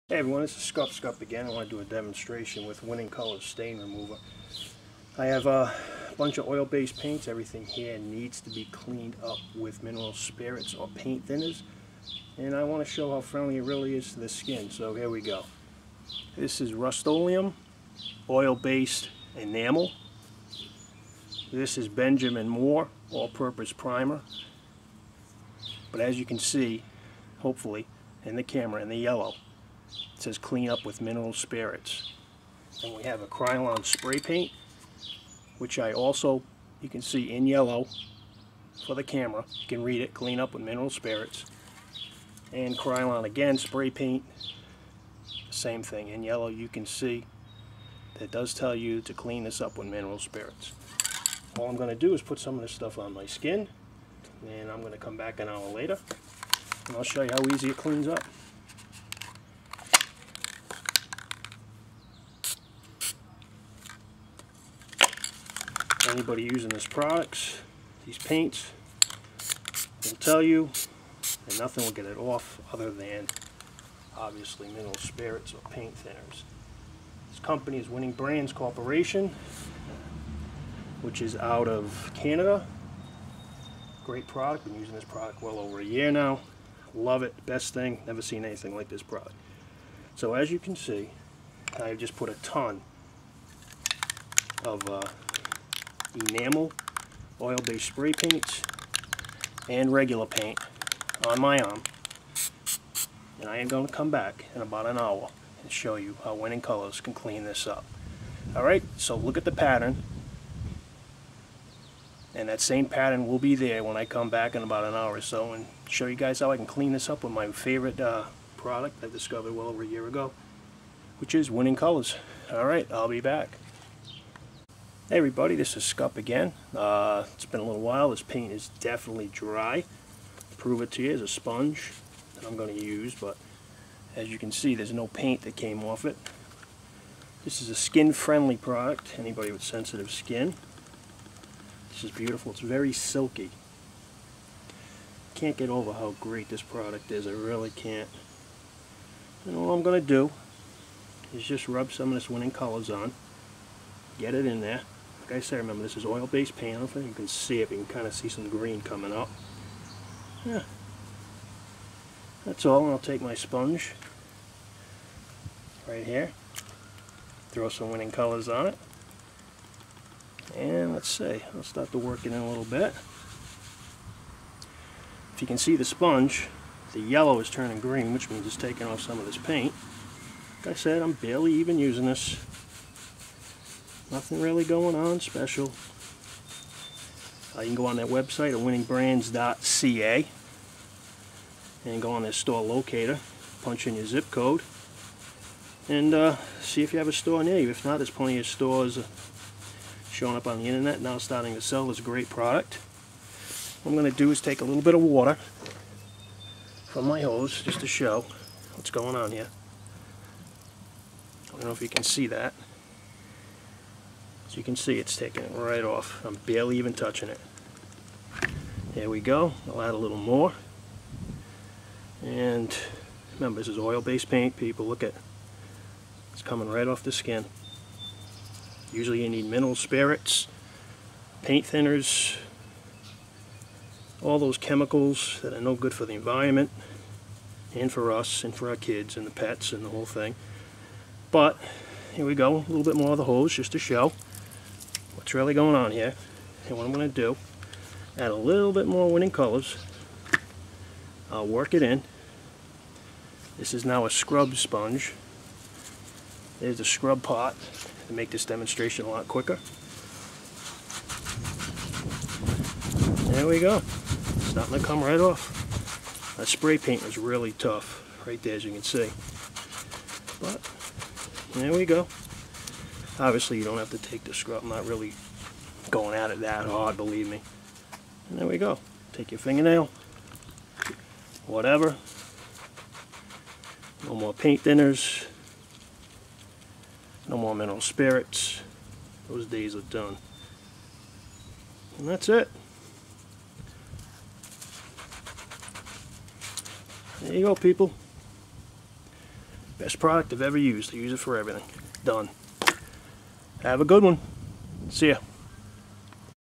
1000+ Stain Remover (Winning Colours), removing artists spray paint from skin
1000+ Stain Remover (Winning Colours) is a safe and effective way to remove artists spray paint from your skin.  See how it works for me.